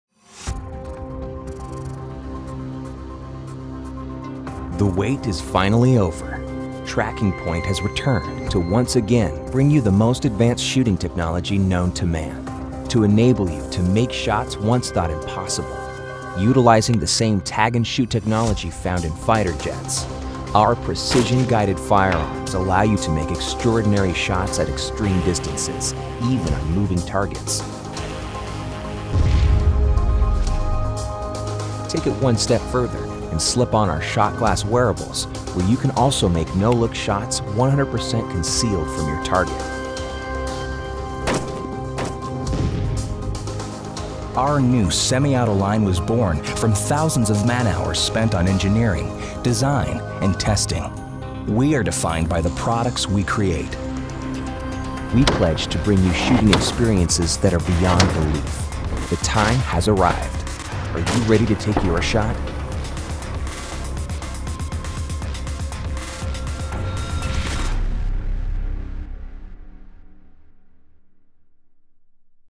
TrackingPoint: The Age of the PGF
Make shots previously thought impossible. TrackingPoint presents the world's first high tech precision guided firearms.